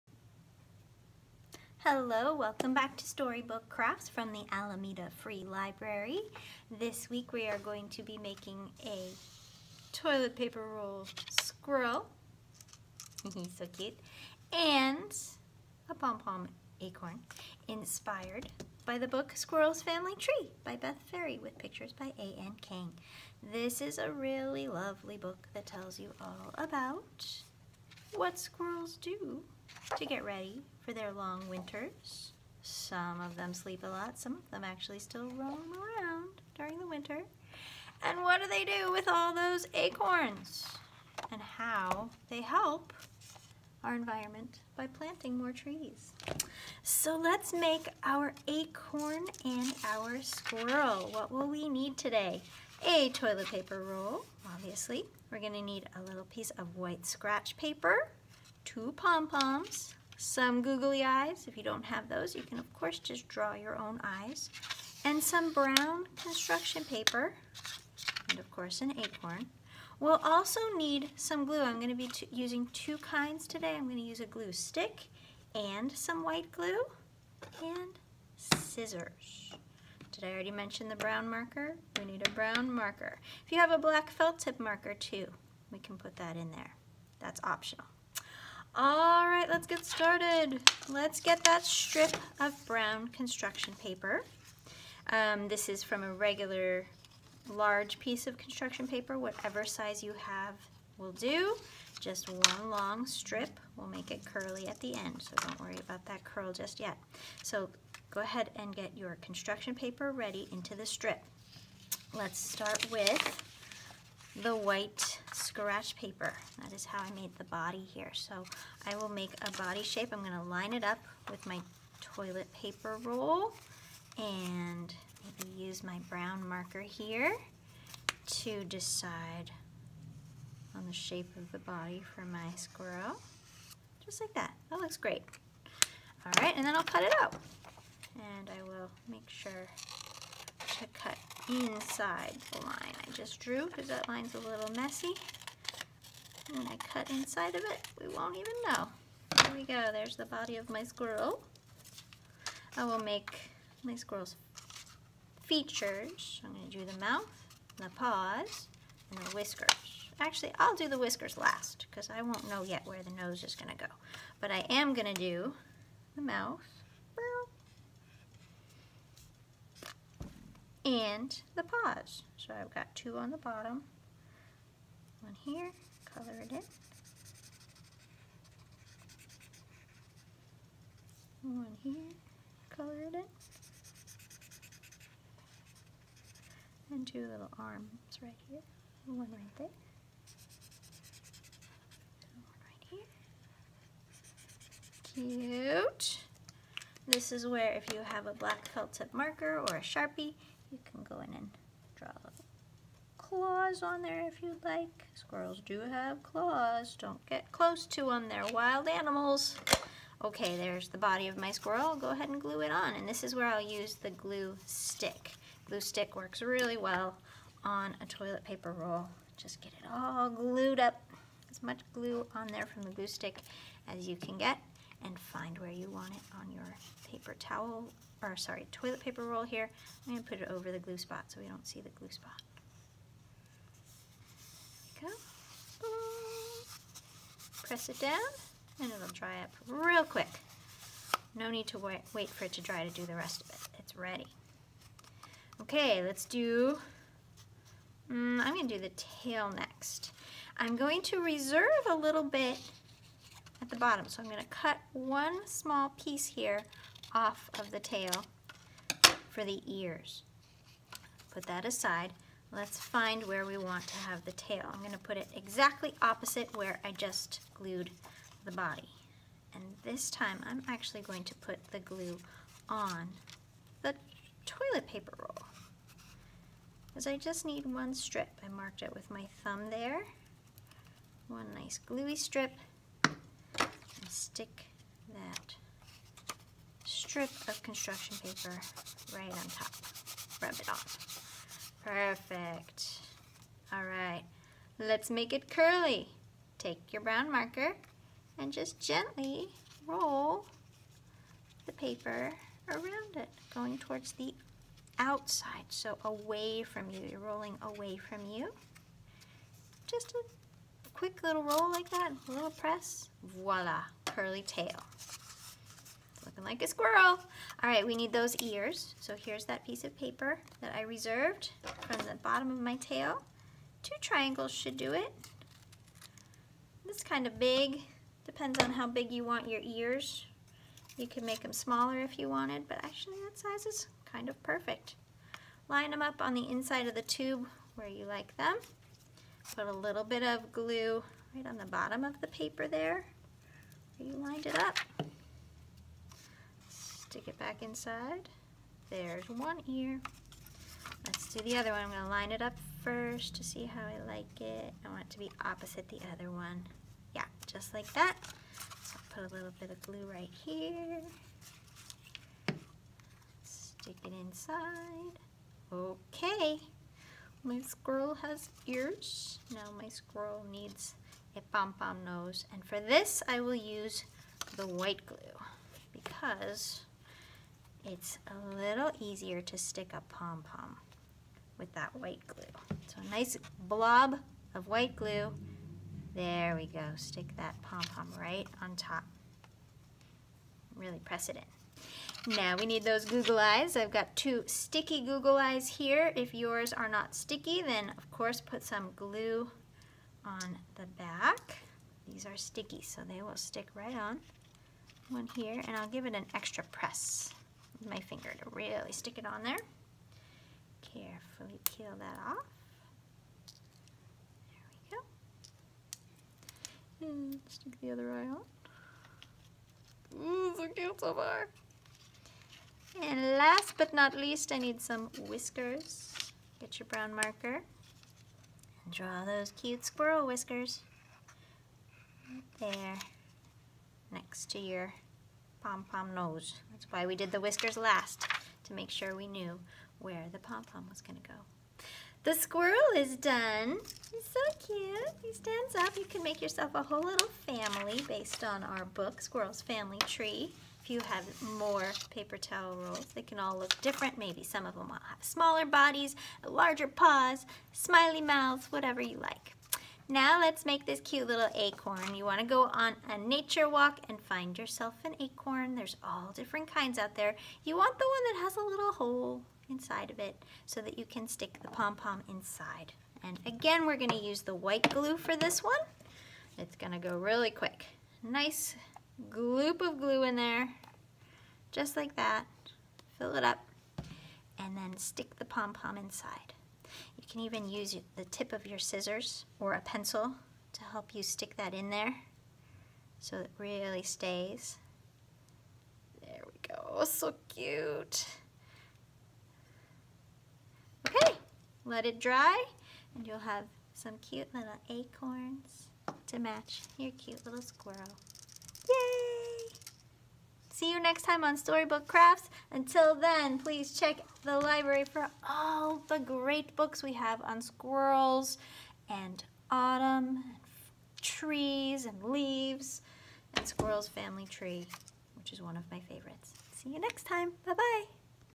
Alameda Free Library Storybook Crafts: Squirrel’s Family Tree Squirrel and Acorn.
Make a tp roll squirrel and a pom pom acorn inspired by the book Squirrel’s Family Tree, by Beth Ferry. You’ll need: a toilet paper roll; a long strip of brown paper; a piece of white scratch paper; two pom poms; a brown marker; a black felt tip pen; an acorn; white glue and a glue stick; and scissors.